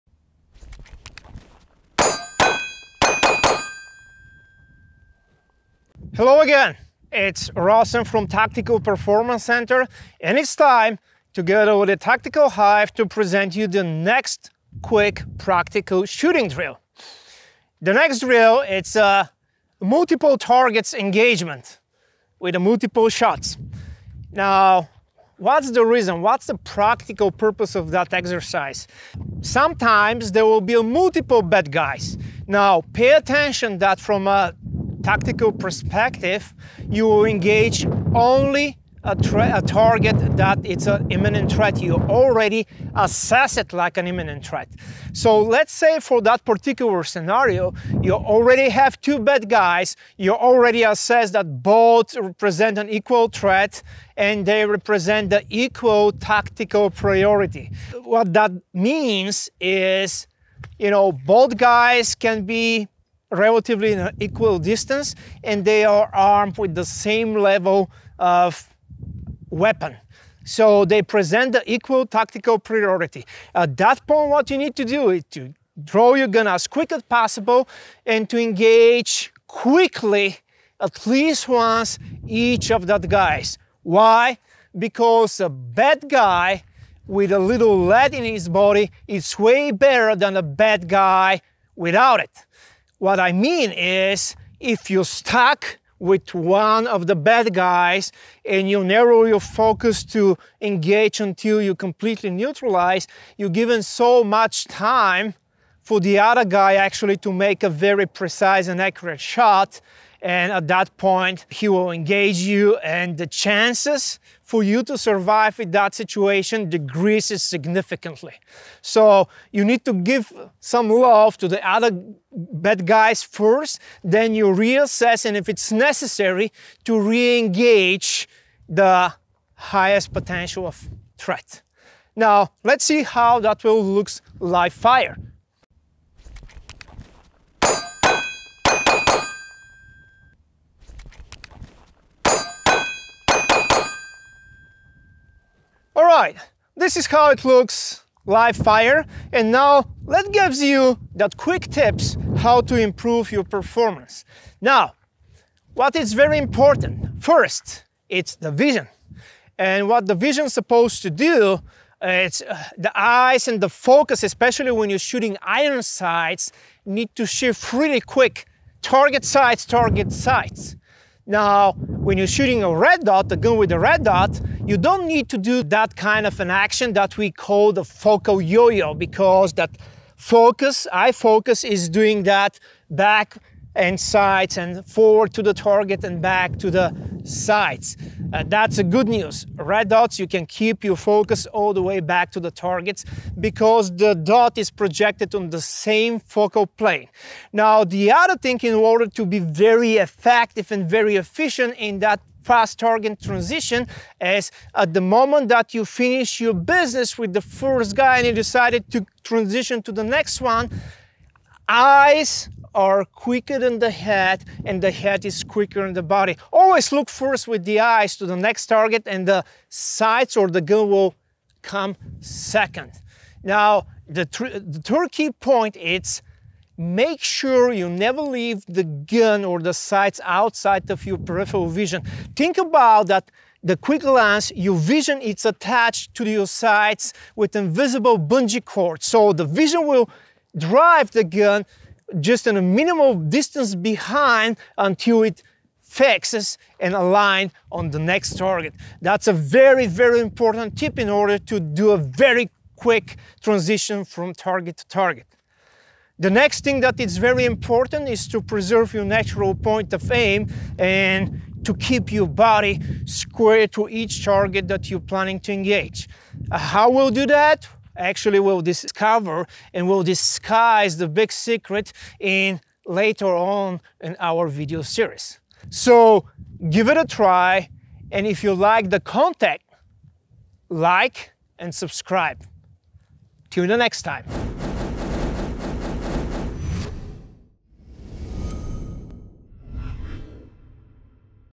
Engaging Multiple Targets More effectively with Tactical Performance Center
In a previous video, we talked about working on target transitions by focusing on a vision exercise.

In this video, Rossen Hristov, Director of Training at Tactical Performance Center and CZ sponsored shooter, shares some additional tips to help with multiple target engagements.

Looking for training in the U.S.? Head over to our directory